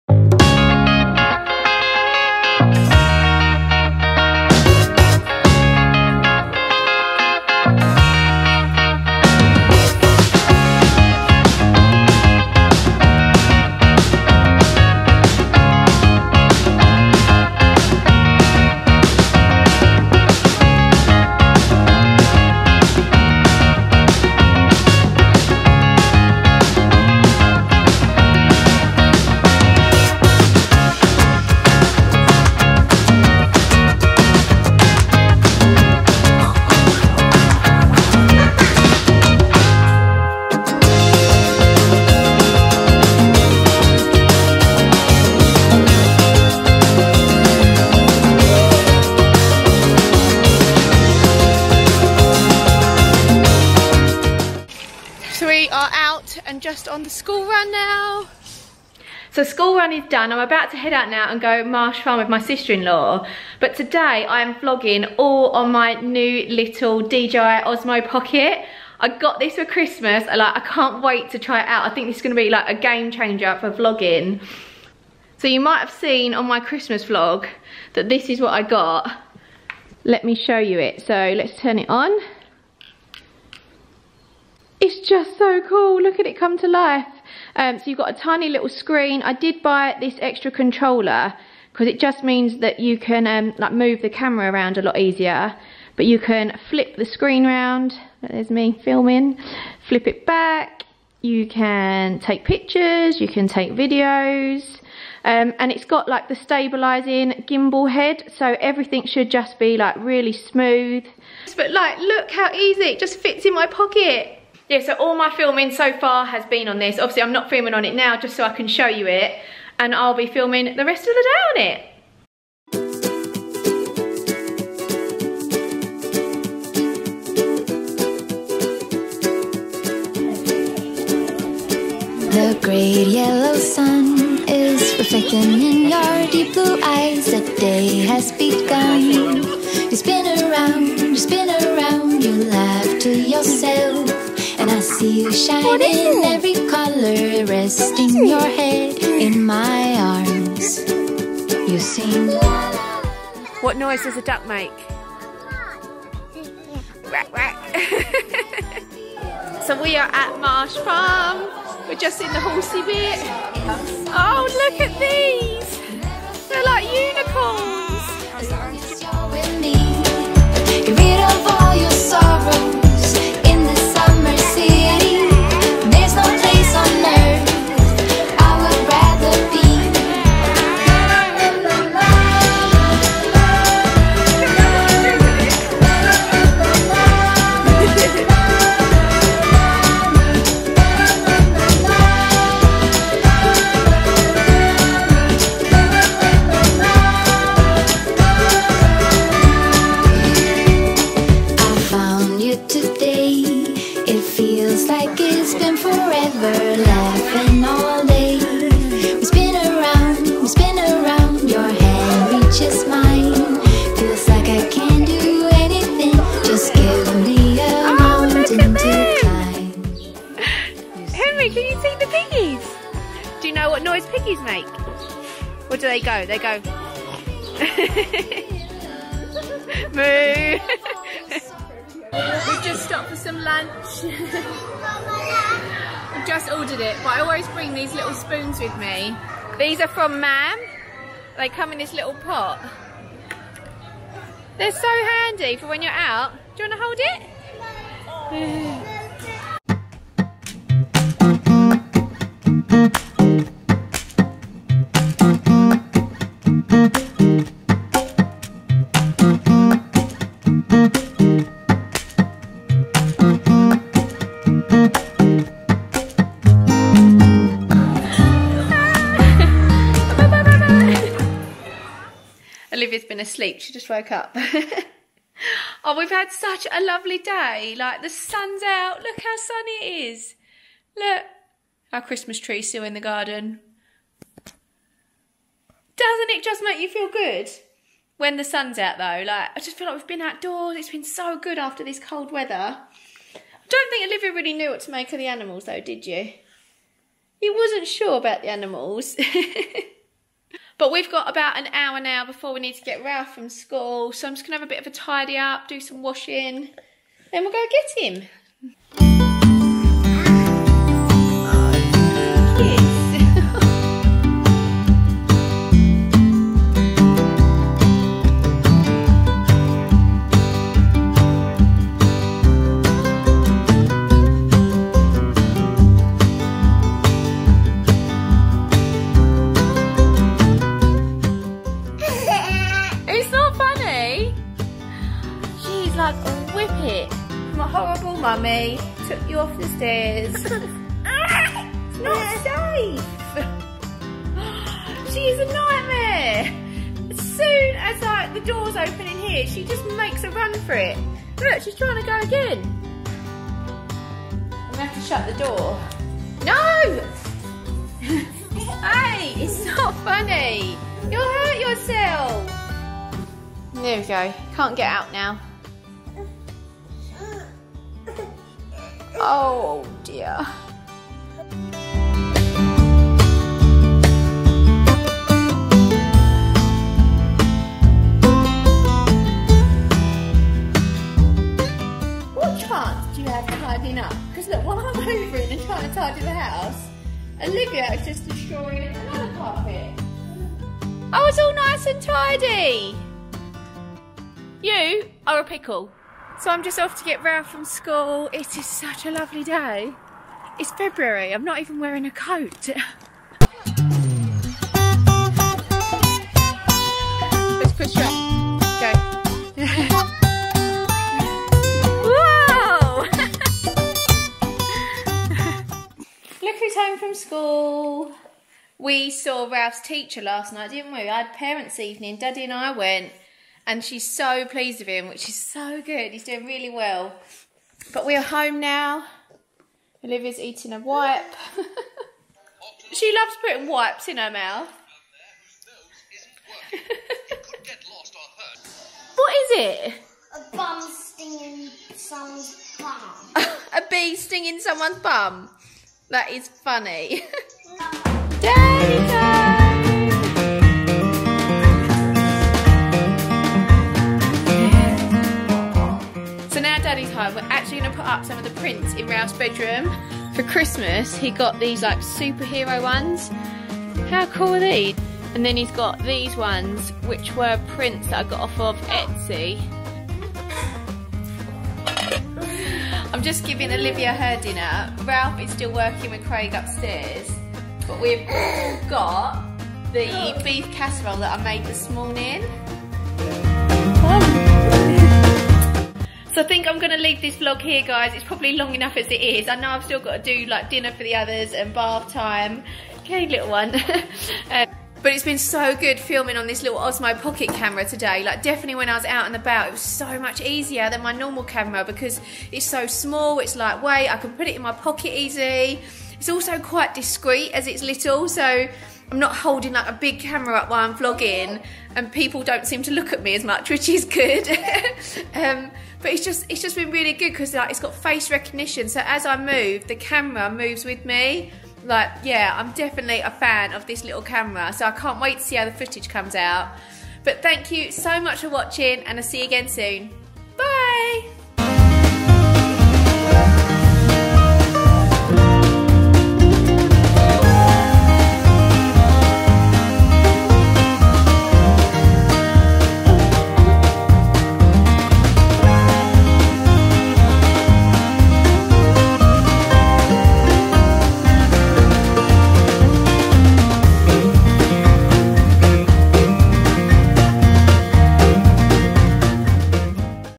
DITL Filming with the DJI OSMO Pocket Camera | Mumlife at the Farm
This video is a day in a life of a mum, I went to Marsh Farm with my baby girl but today I was filming with my DJI OSMO Pocket camera.